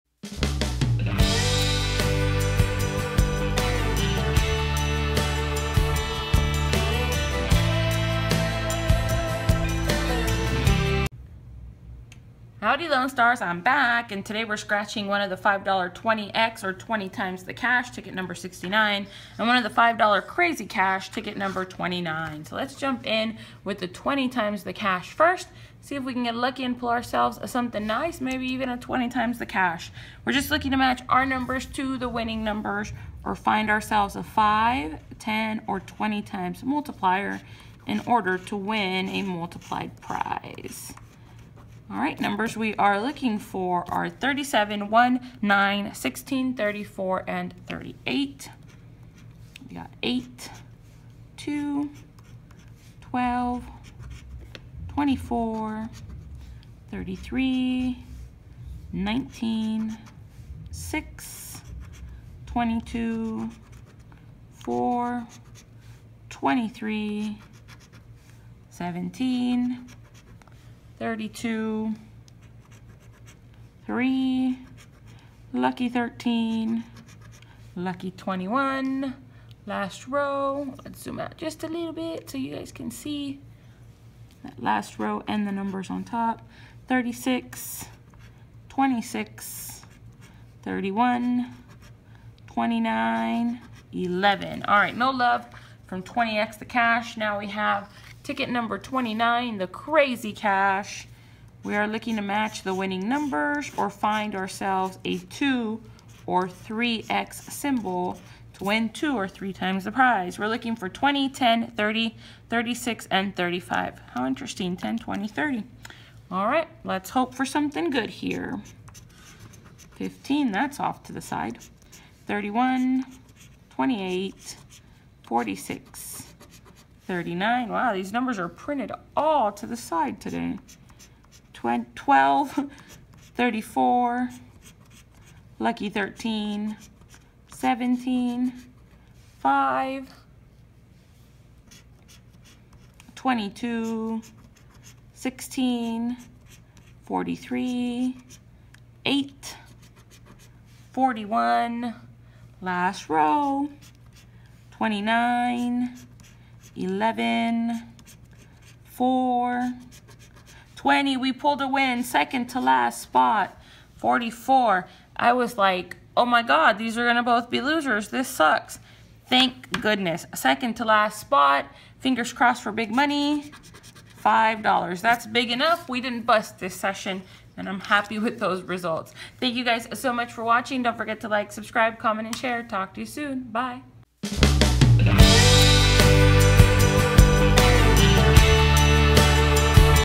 WIN! $5 20X The Cash & $5 Crazy Cash Texas Lottery Scratch Off Tickets
Scratching the $5 20X The Cash & $5 Crazy Cash Texas Lottery Scratch Off Tickets. Will I find a big win?
Fan Mail:
Candy